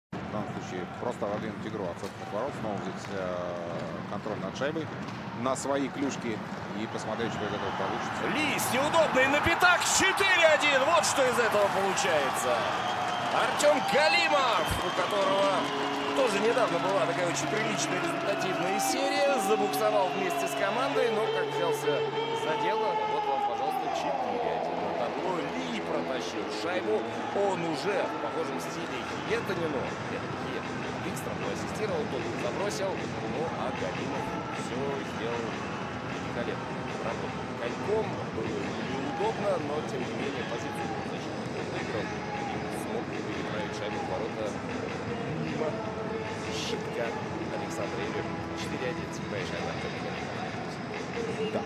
Artyom Galimov sick goal
Ak Bars rookie Artyom Galimov scores yet another goal - this time assisted by Oleg Li